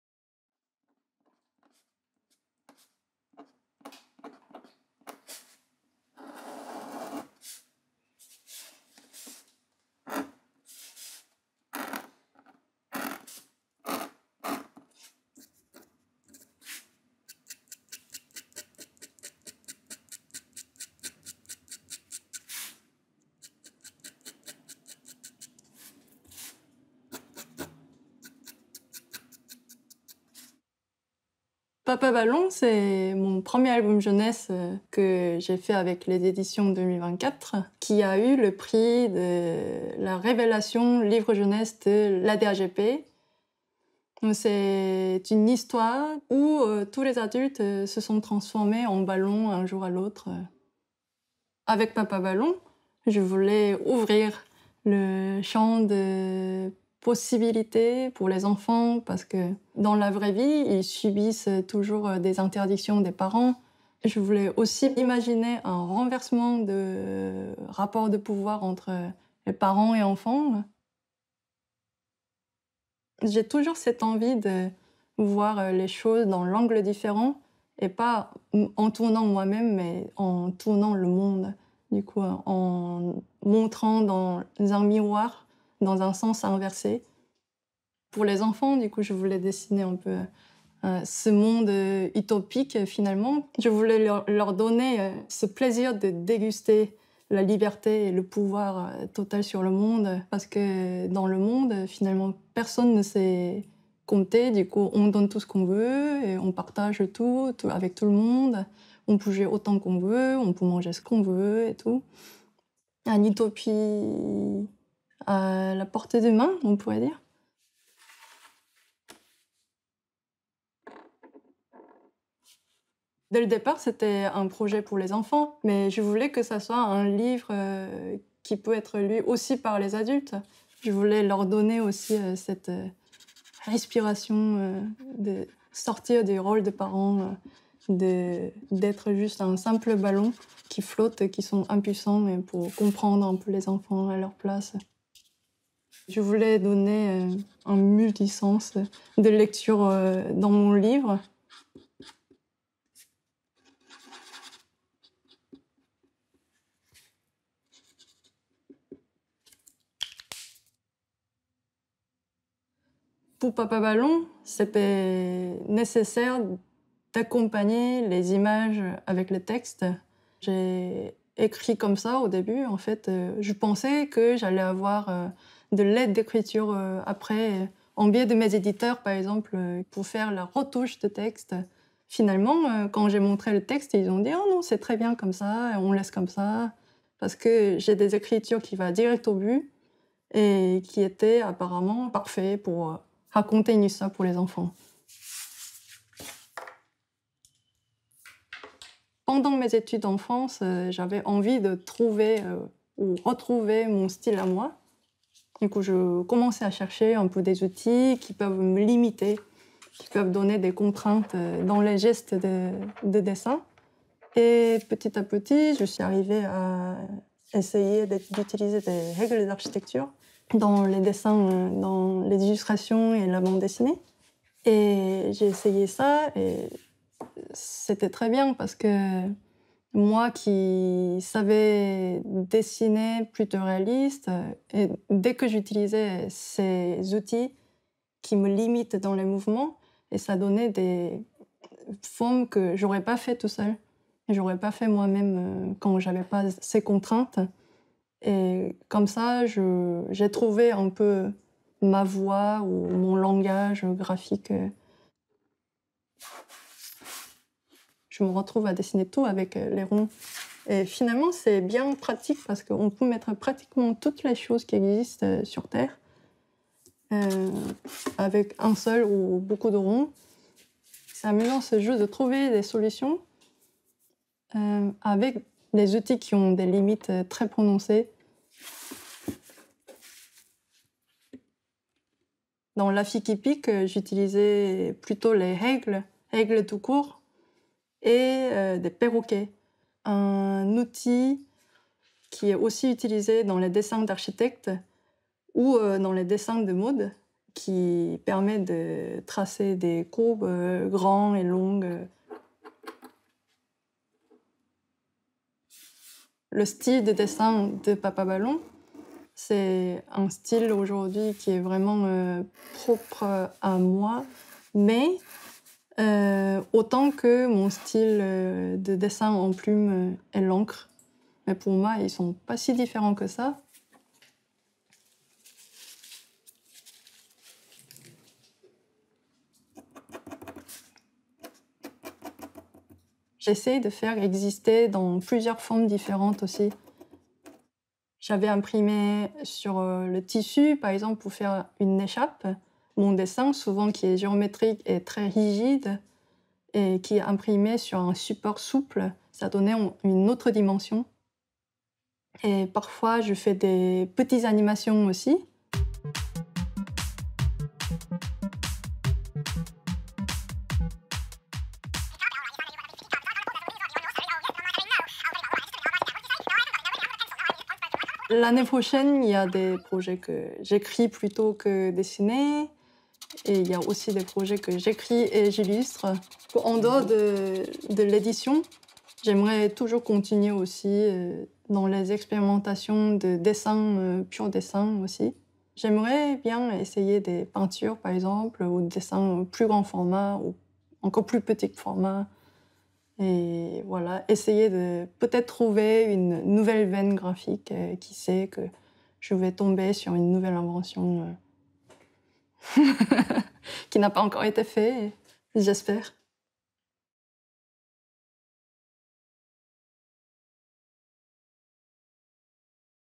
Saehan PARC, Révélation Livre Jeunesse 2021 - Atelier A / ADAGP
Entrez dans les ateliers des artistes avec les Portraits Atelier A / ADAGP !

Cette semaine, Saehan PARC est l’artiste mise à l’honneur.
Lauréate de la Révélation Livre Jeunesse 2021 ADAGP / Charte des auteurs et des illustrateurs, son premier ouvrage « Papa Ballon » est paru en avril 2021 aux éditions 2024.

Suivez-nous !